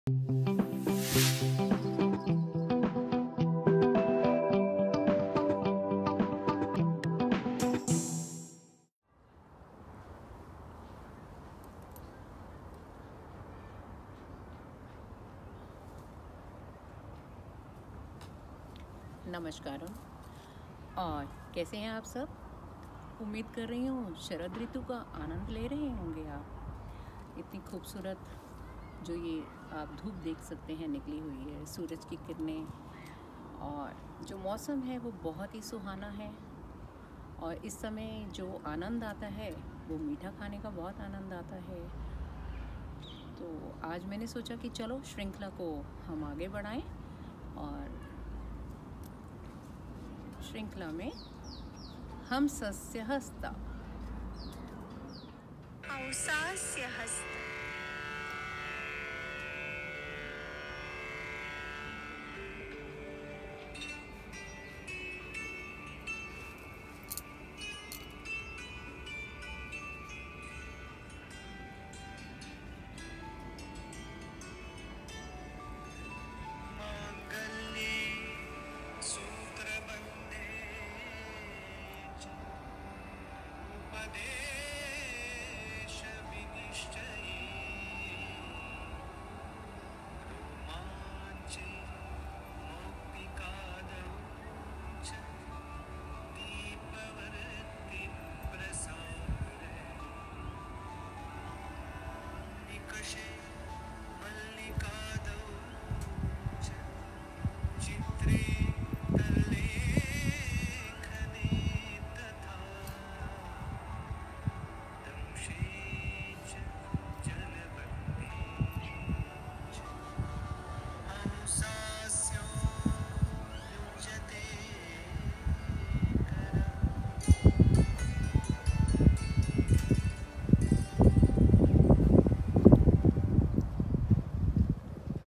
Learn Bharatnatyam ( Sudarshan Natessha)
Hamsāsya Hasta ( swan Beak ) , If the thumb and the fore finger touch each other and the remaining fingers are out stretched the hand is called Hamsāsya Hasta . Uses :Blessings or festival, the tying with thread , ascertaining instruction, pearls, putting forward to wick of a lamp , a touchstone, a jasmine, a painting, the act of painting and a dyke impending a current. From Abhinaya Darpana.. Asamyukta Hastas .to be Continued